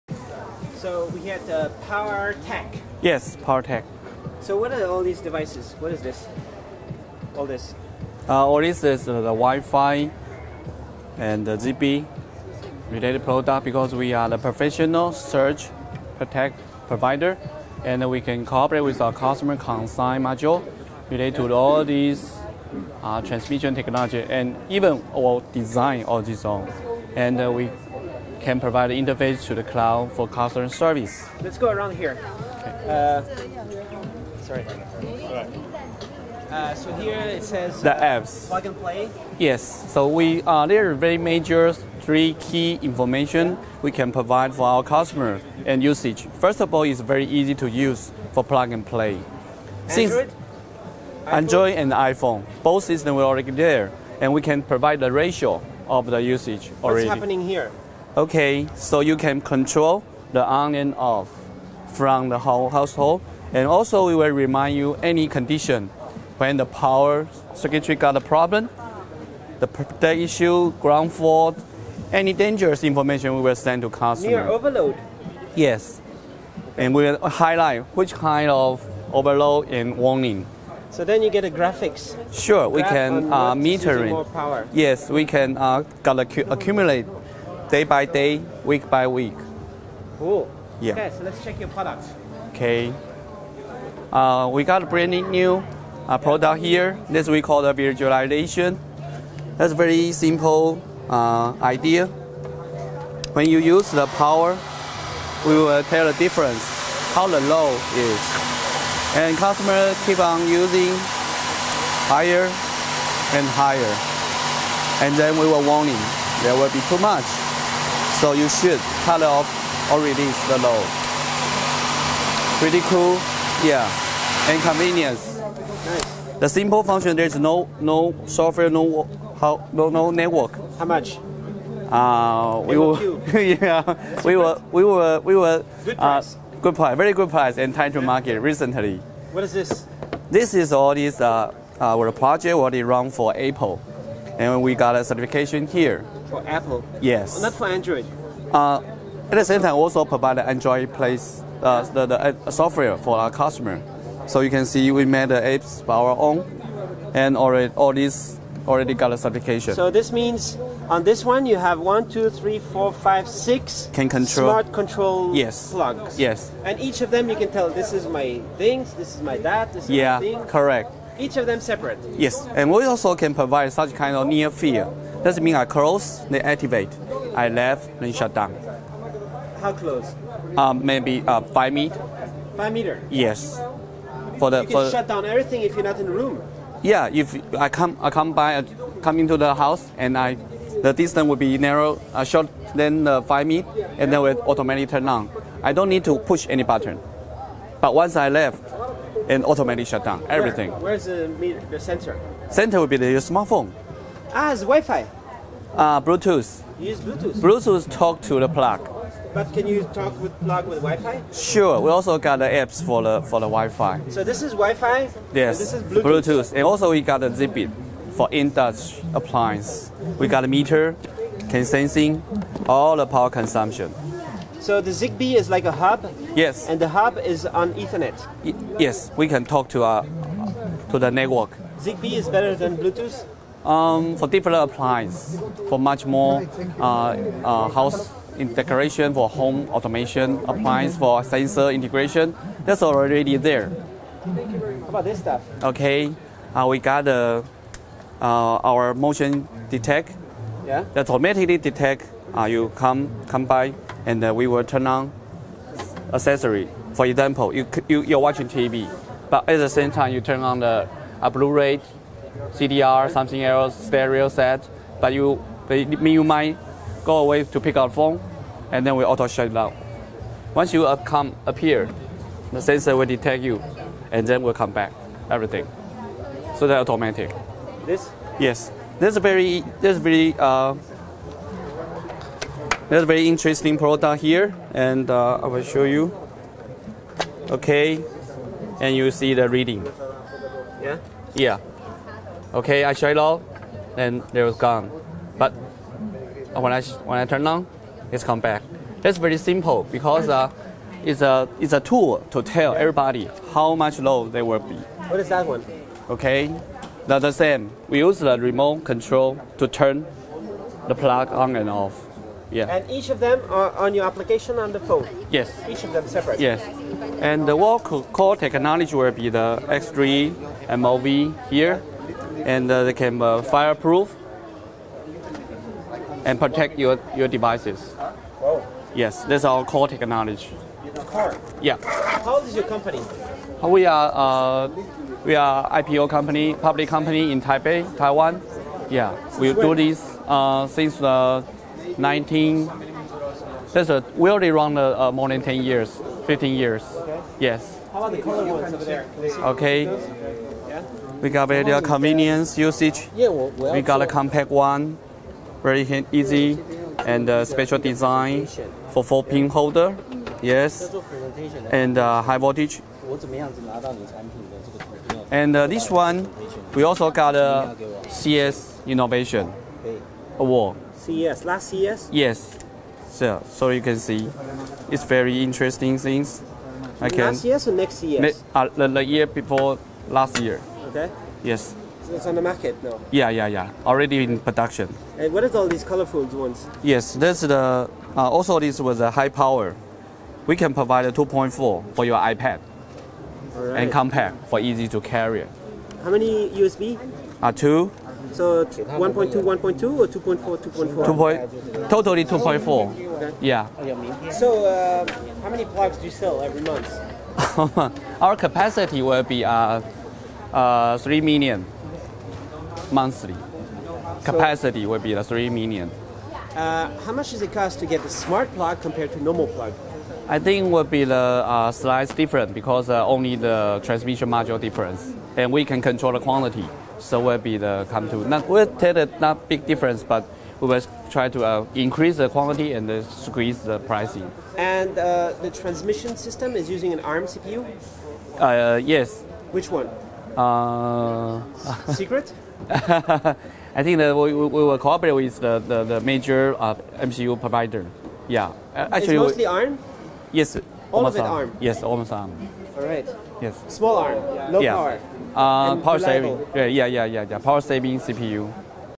Powertech Smart Multi Plug Adapter, Motion Detector, Light Meter and more
Power Tech can provide Plug &amp; Play Controls, Power Safety and Power Usage information Applications for Android or iOS system.

Power Tech make Power Plugs that have an LED light that warns you when you are over-using the power capacity of the Plug to prevent future damages to the environment.

They make Smart Multi Plug Adapter with a transmission module. Each Plug can be controlled from your Android or iOS device. These Plugs have an option that can be turned on or off when the person is near the central Plug in the room.

Motion Detector, Usb charging adapter and Light Meter are other products that get made from Power Tech.

Power Tech Company is based in Taiwan.

Filmed at Filmed at the HKTDC Hong Kong Electronics Fair (Autumn Edition) 2013 using the Tiffen Steadicam Merlin 2 with arm and vest